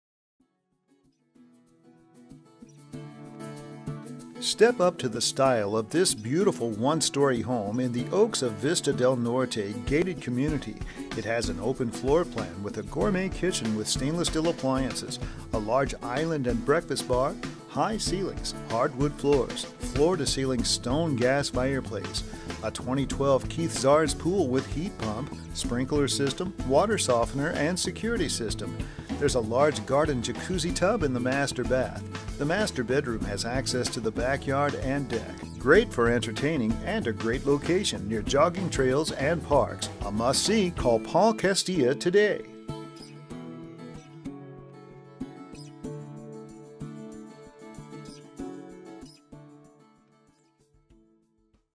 13003 VIDORRA VISTA DR, San Antonio, TX 78216
Beautiful one-story home in The Oaks of Vista Del Norte gated neighborhood. Open floor plan, gourmet kitchen with stainless steel appliances, large island/breakfast bar, high ceilings, hardwood floors, floor to ceiling stone gas fireplace, 2012 Keith Zars pool w/heat pump, sprinkler system, water softener, security system. Large garden jacuzzi tub in master bath, master bedroom has access to back yard and deck. Great for entertaining and great location near jogging trails and parks a must-see.